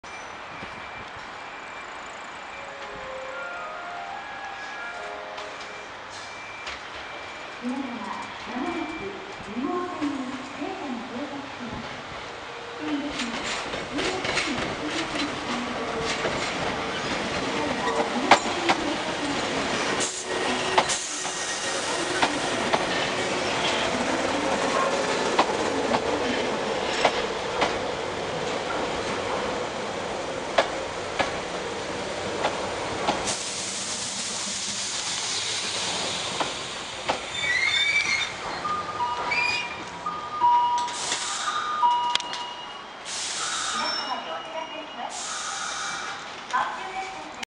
2016.12.09 阪急 3000系 3052F 普通 伊丹 行き 到着 伊丹駅 阪急電車 阪急電鉄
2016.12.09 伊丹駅
3052F 4両編成
(Tc)3052-(M)3501-(T)3651-(M'c)3007